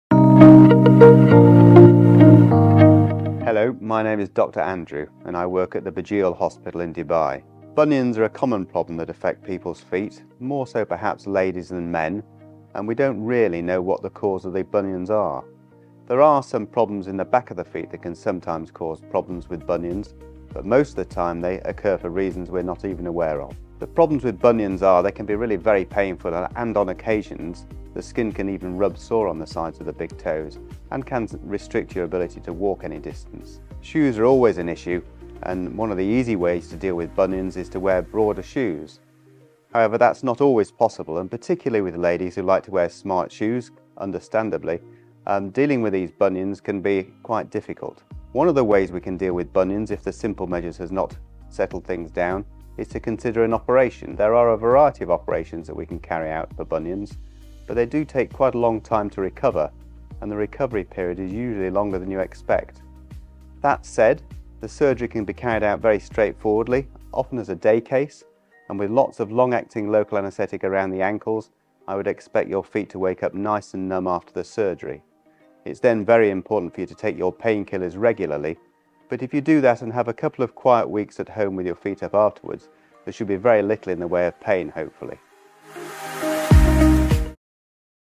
Dr. Andrew Foggitt Unveils Bunion Treatment Breakthroughs | Comprehensive Guide by Burjeel Dubai
Join Dr. Andrew Charles Foggitt, a renowned Consultant Orthopedic Surgeon from Burjeel Hospital for Advanced Surgery, Dubai, as he dives deep into the world of bunion care. In this exclusive and enlightening video, Dr. Foggitt uncovers the complexities behind bunions - a common yet misunderstood foot condition. Discover the latest insights into the causes, complications, and cutting-edge surgical solutions for bunions.

🔹 About Dr. Andrew Foggitt 🔹
Dr. Foggitt brings his extensive expertise to the forefront, offering valuable advice and information that is crucial for anyone suffering from bunions or foot-related issues.